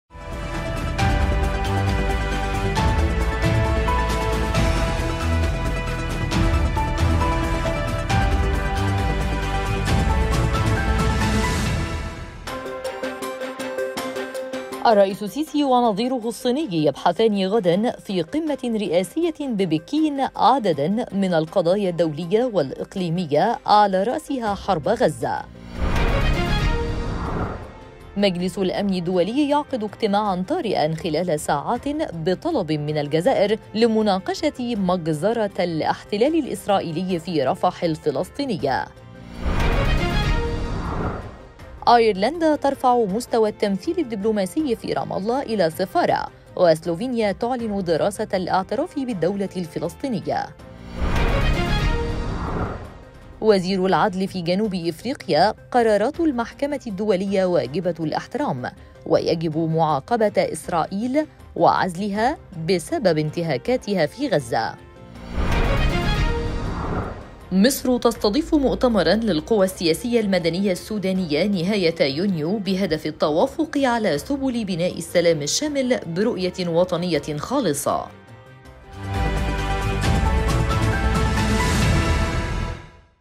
دقيقة أخبار | أيرلندا ترفع مستوى التمثيل الدبلوماسي في رام الله إلى سفارة
دقيقة أخبار | أيرلندا ترفعُ مستوَى التمثيلِ الدبلوماسيِّ فِي رام الله إلَى سفارة.. وسلوفينيا تعلنُ دراسَةَ الاعترافِ بالدولةِ الفلسطينية

الرئيسُ السيسي ونظيرُهُ الصينيُّ يبحثانِ غدًا في قمةٍ رئاسيةٍ ببكينَ عددًا من القضايَا الدوليةِ والإقليميةِ علَى رأسِهَا حربَ غزة

مجلسُ الأمنِ الدوليِّ يعقدُ اجتماعًا طارئًا خلالَ ساعاتٍ بطلبٍ منَ الجزائِرِ لمناقشةِ مجزرَةَ الاحتلالِ الإسرائيليِّ فِي رفحَ الفلسطينية

أيرلندا ترفعُ مستوَى التمثيلِ الدبلوماسيِّ فِي رام الله إلَى سفارة.. وسلوفينيا تعلنُ دراسَةَ الاعترافِ بالدولةِ الفلسطينية

وزيرُ العدلِ فِي جنوبِ إفريقيَا: قراراتُ المحكمةِ الدوليةِ واجبةُ الاحترام.. ويجبُ معاقبةَ إسرائيلَ وعزلِهَا بسببِ انتهاكاتِهَا في غزة

مصرُ تستضيفُ مؤتمرًا للقوَى السياسيةِ المدنيةِ السودانيةِ نهايةَ يونيو بهدفِ التوافُقِ علَى سُبُلِ بناءِ السلامِ الشاملِ برؤيةٍ وطنيةٍ خالصة